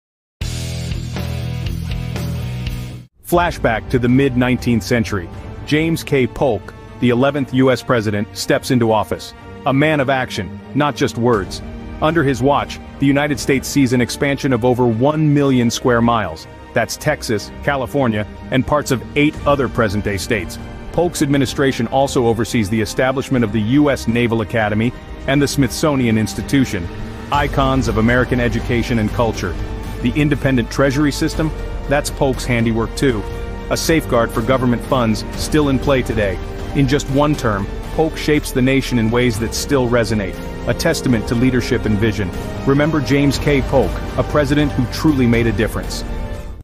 President James Polk: His Power Plays
Learn about the life and presidency of James K. Polk, the 11th President of the United States, in this YouTube short. Discover key facts, accomplishments, and historical significance of this often underrated leader.